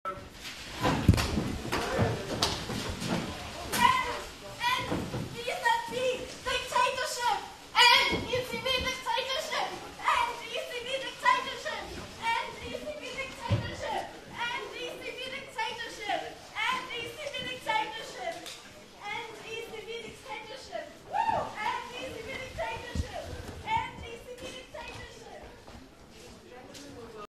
ECBドラギ総裁の会見に女性が乱入
ECB President Mario Draghi attacked by protester named Josephine Witt